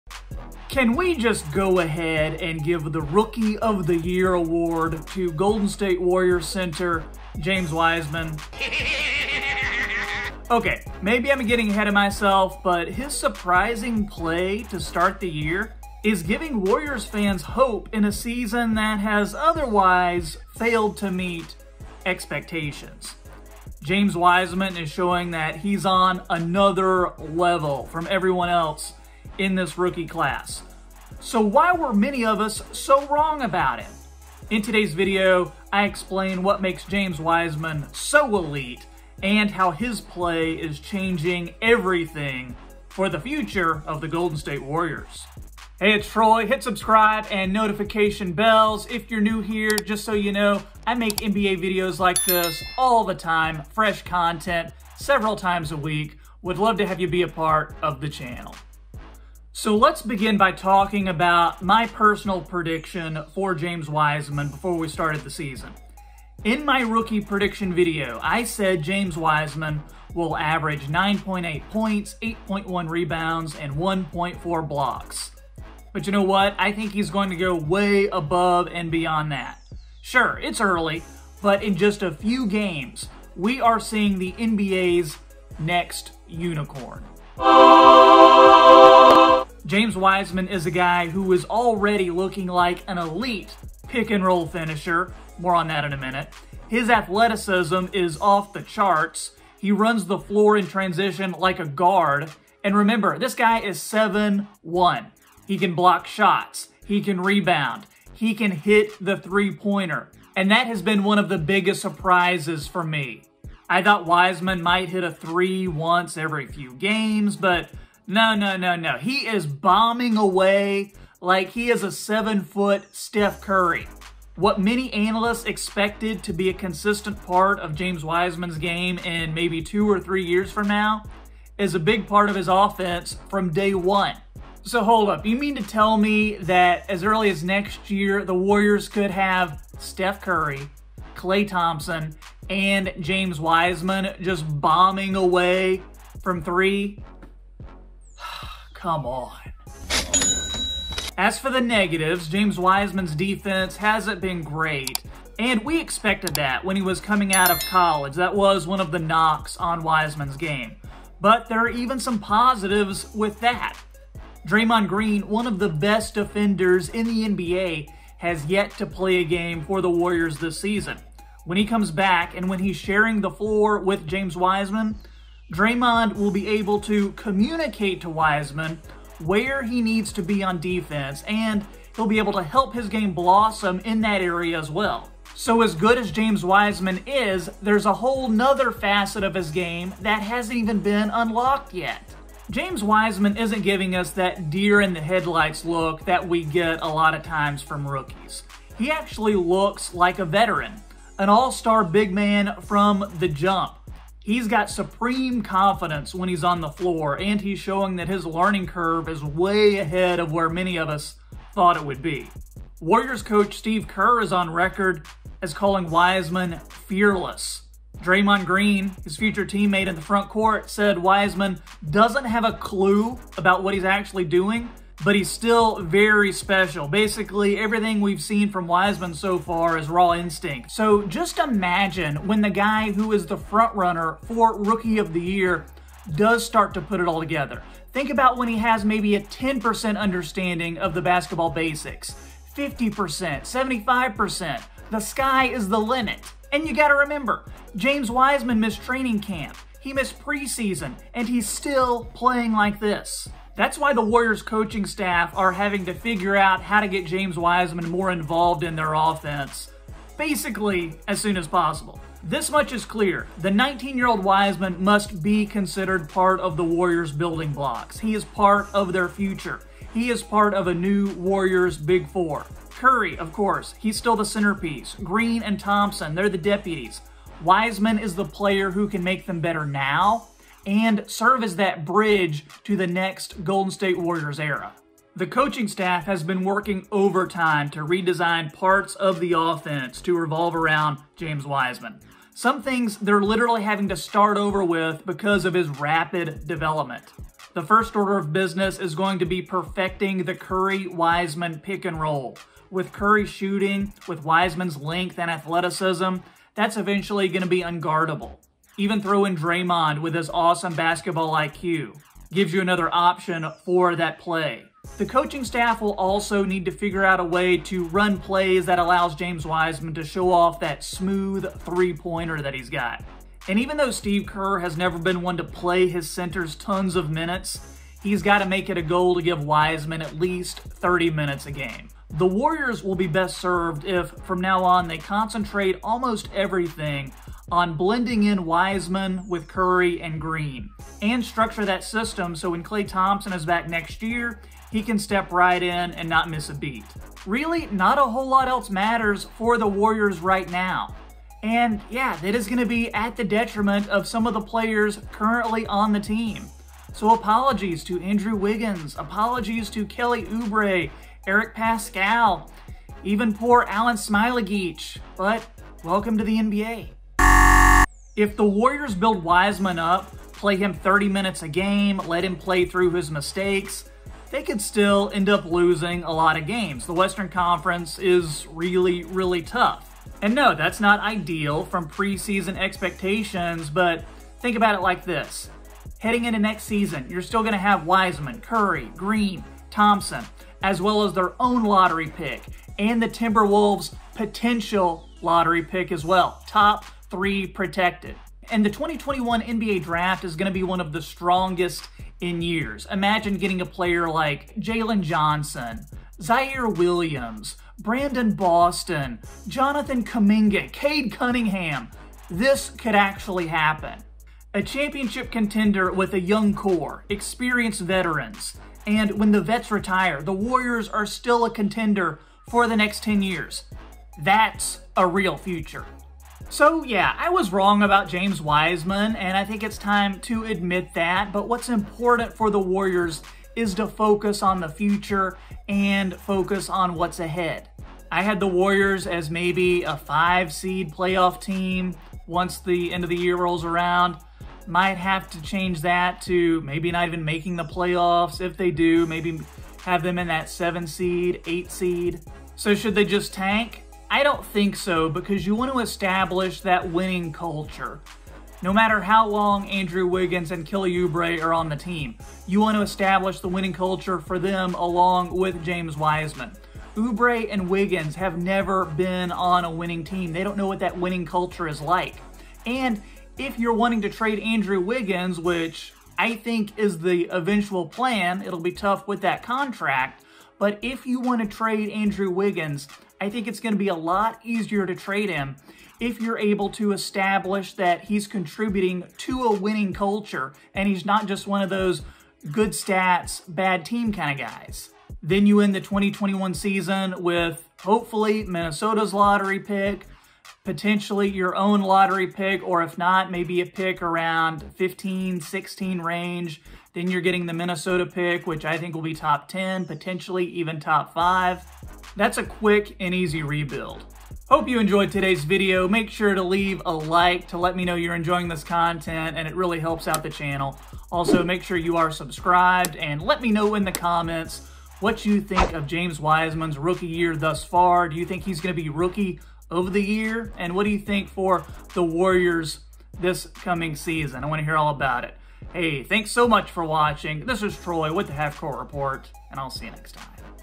James Wiseman Is Proving Everyone WRONG! [A LOCK For Rookie of the Year?]
Golden State Warriors rookie James Wiseman has impressed in his first few NBA games. His chemistry with Stephen Curry is already apparent. This video examines how good Wiseman can be this season and beyond, and reacts to the amazing start for James Wiseman and what that means for the Warriors dynasty.

► Make sure you LIKE this video and COMMENT ANYTHING so YouTube recommends it to others!